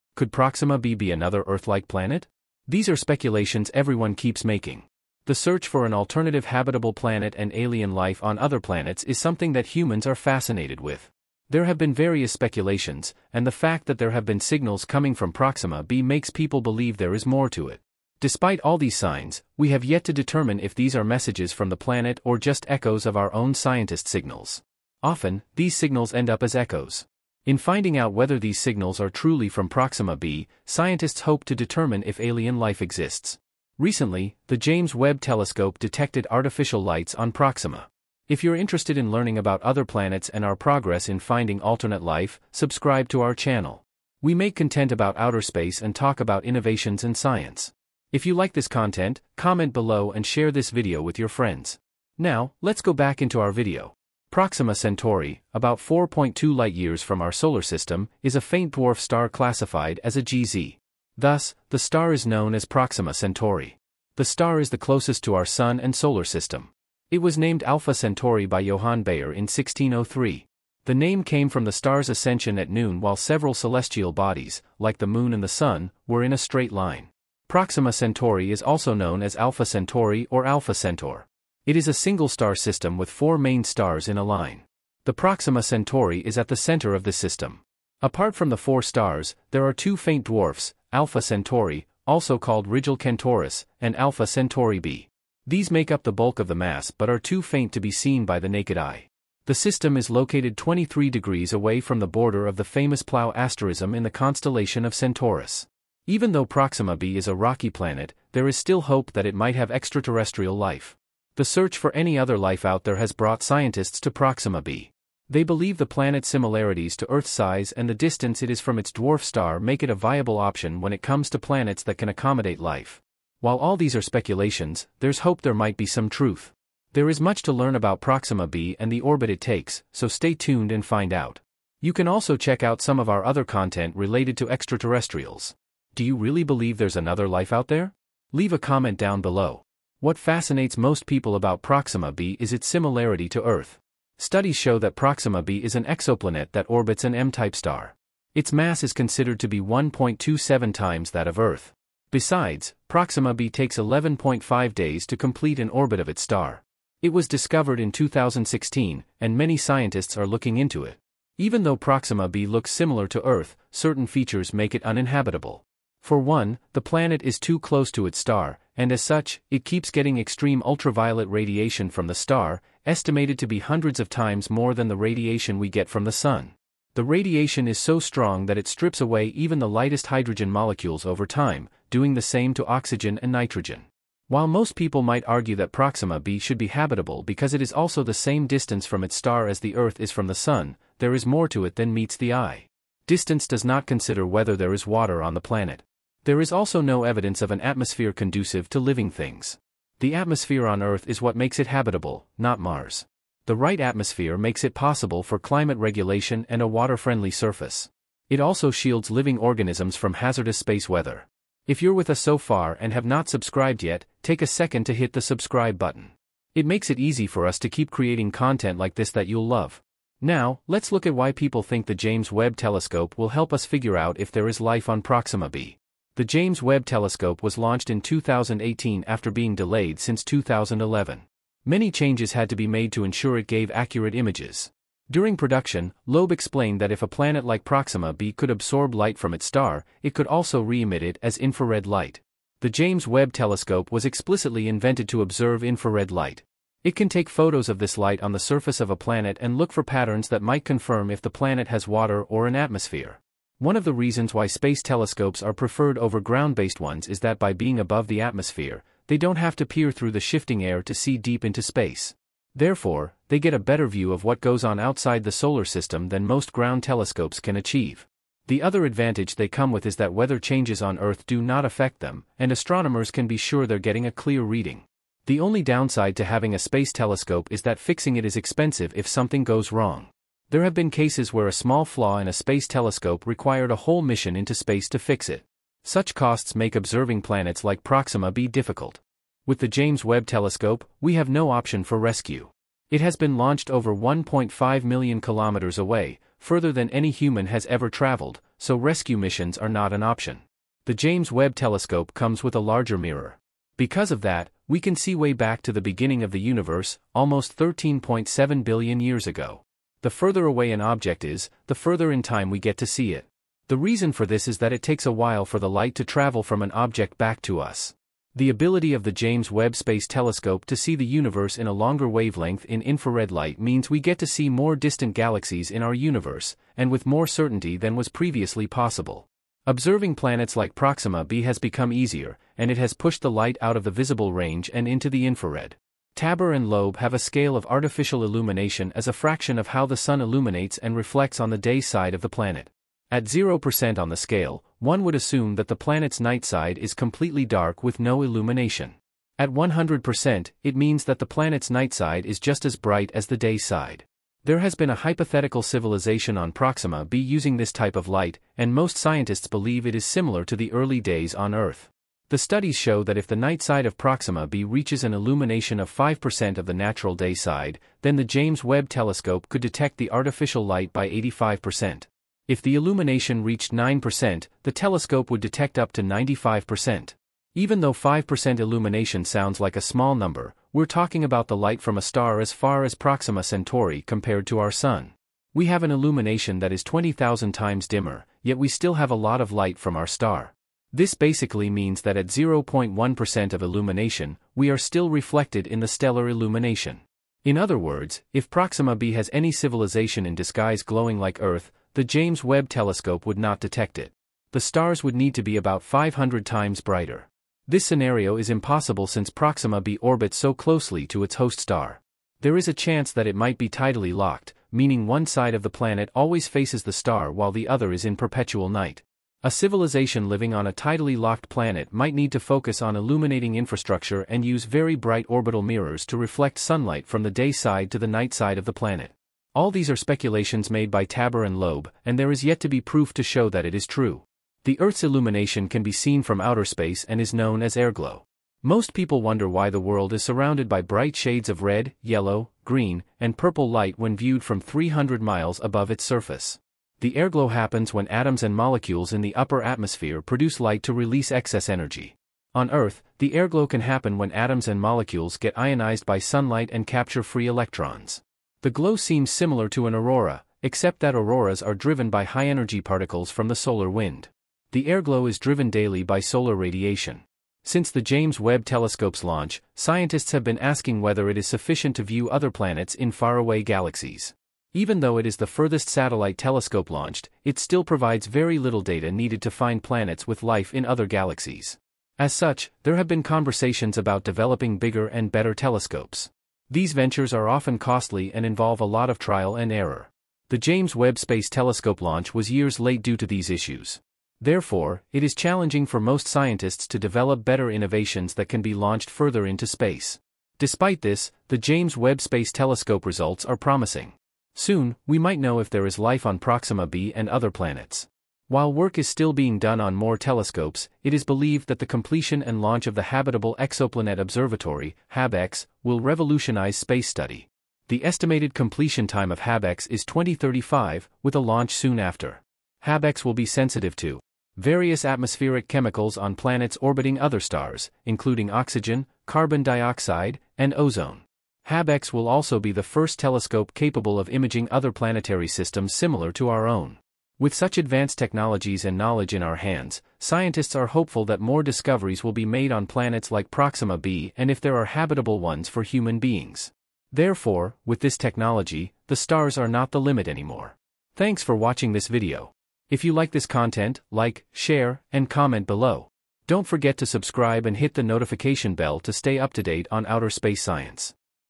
2 MINUTES AGO: James Webb Telescope Discovers Terrifying City Lights on Proxima B!
🚨 BREAKING NEWS: The James Webb Space Telescope has just detected mysterious city-like lights on Proxima B, an exoplanet orbiting our closest star, Proxima Centauri. Could this be proof of an advanced alien civilization? What do these strange lights mean, and why is this discovery so alarming?